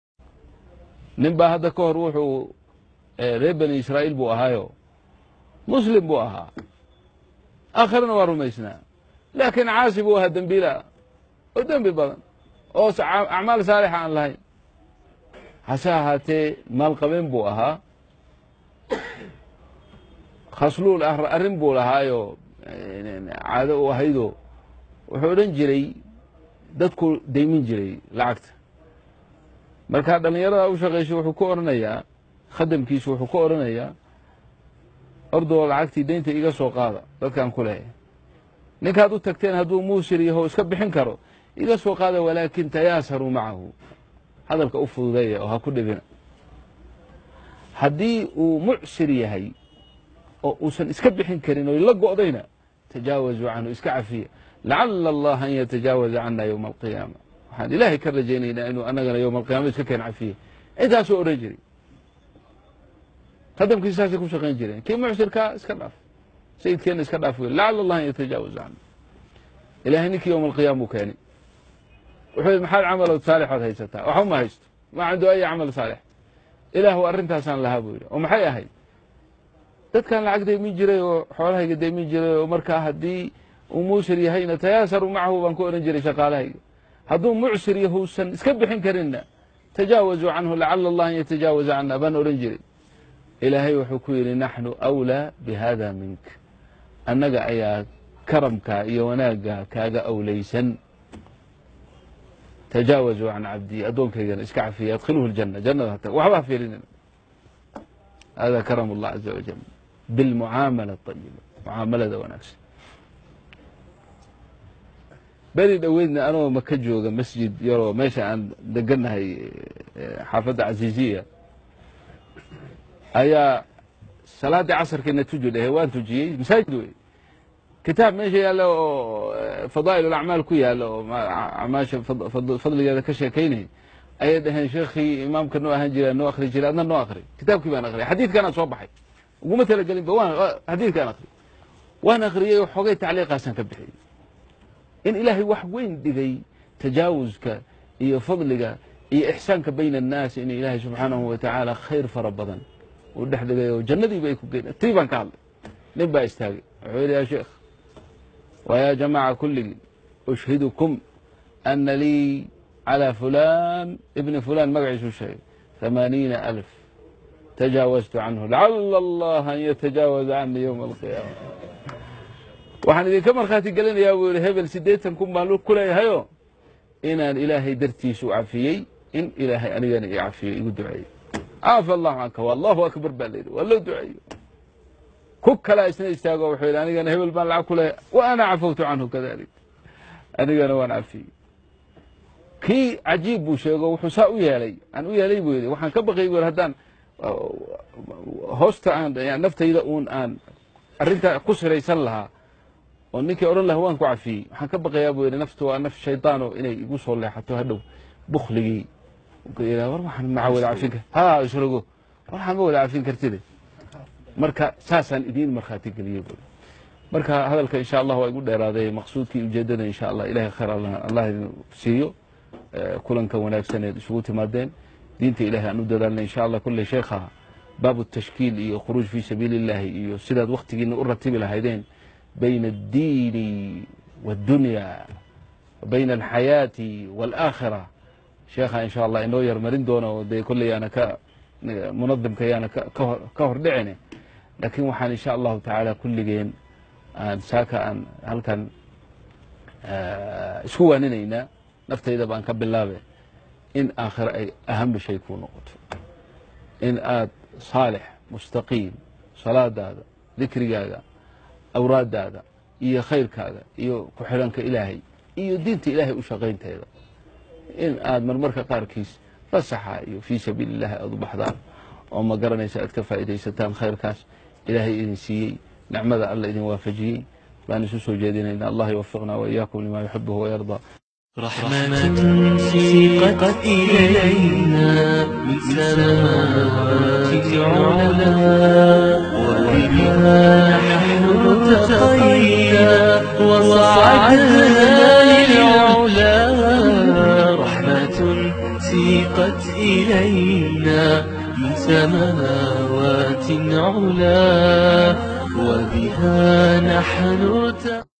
Cafiska iyo qiimaha uu kuuleeyahay hadii aad dadka cafiso sh cabdirashid sh sufi
Cafiska iyo qiimaha uu kuuleeyahay hadii aad dadka cafiso ood mucaamulo wanaagsan kula dhaqanto illahayba adiga kucafinaya akhariyo aduunba cafiska iyo mucaamulada wanaagsan halagu dadaalo daawo muxaadaro cafiska iyo mucaamulada fiican kusaabsan sheikh cabdi rashiid ali suufi ayaa soo jeedinaya illahay khayr hasiiye daawosho wacan shere gareer